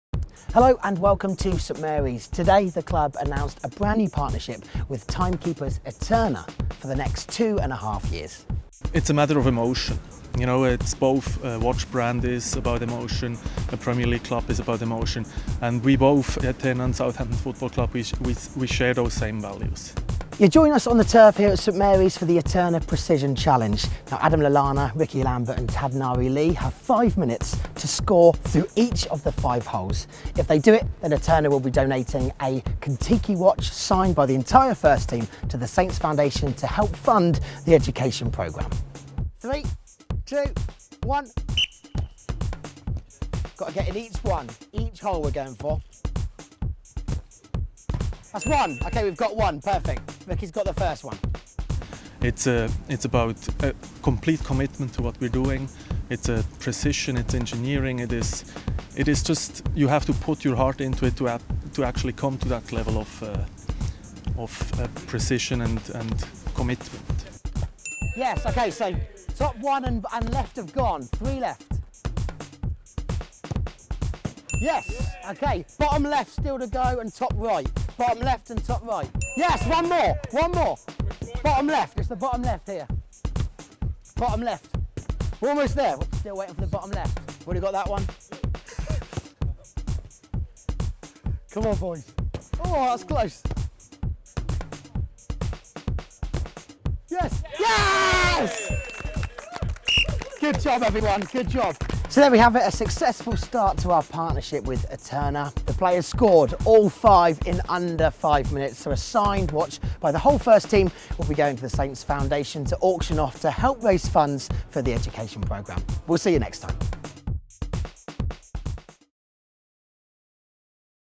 The Eterna Precision Challenge
Following Southampton Football Club's announcement of Eterna as our Official Timekeeper, Rickie Lambert, Adam Lallana and Tadanari Lee take part in the Eterna Precision Challenge.